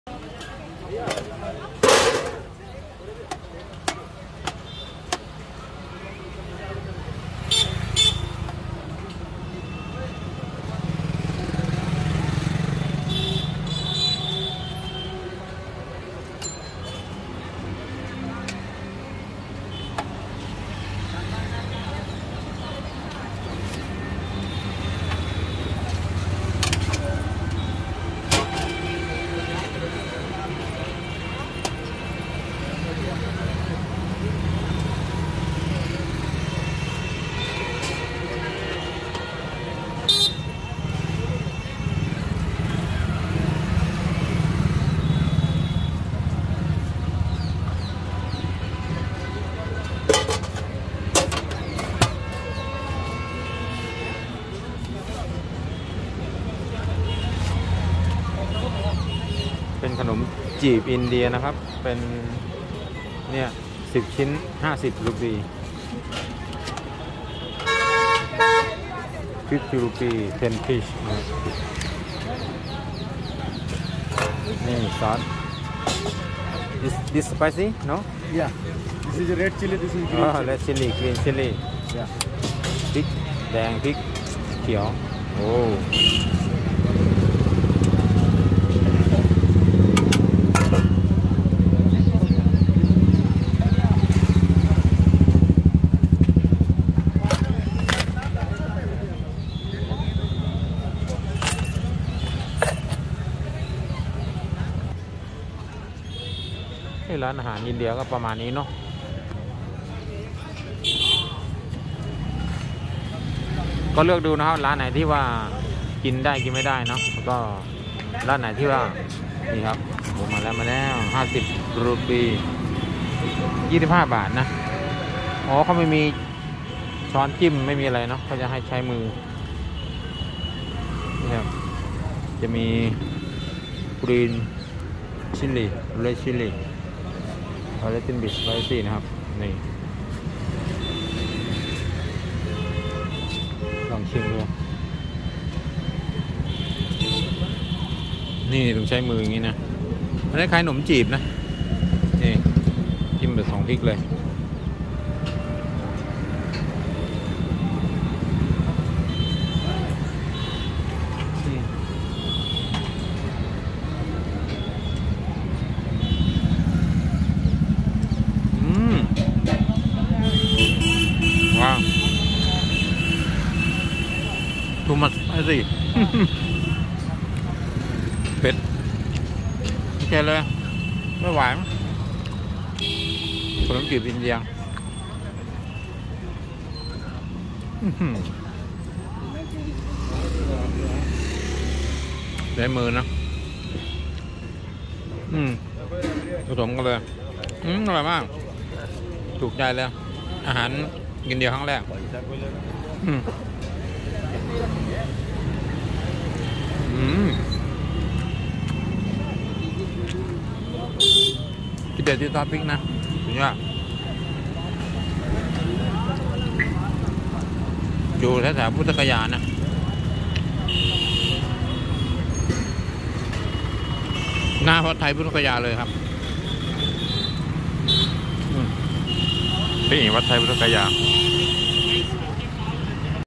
พริกบังเผ็ดจริง ขนมจีบอินเดียสูตร2พริก พริกแดงพริกเขียว 10ชิ้น50รูปี 25 บาท Street Food Gaya India
Momo หรือที่อ่านว่า โมโม่ ลักษณะคล้ายเกี๊ยว หรือเสี่ยวหลงเปาที่เราคุ้นตา เพราะเป็นเมนูที่ได้รับอิทธิพลจากหลากหลายชาติ ทิเบต จีน เนปาล นั่นเองค่ะ ตัวโมโม่นี้จะมีทั้งแบบทอด แบบนึ่ง และแบบผัด สอดไส้ด้วยผักหรือเนื้อสัตว์สับ รสชาติอร่อยกลมกล่อม สามารถจิ้มซอสทานเล่นเป็นอาหารว่าง หรือจะทานคู่กับข้าวก็ได้
พริกบังเผ็ดจริง ขนมจีบอินเดียสูตร2พริก พริกแดงพริกเขียว 10ชิ้น50รูปี 25 บาท Street Food India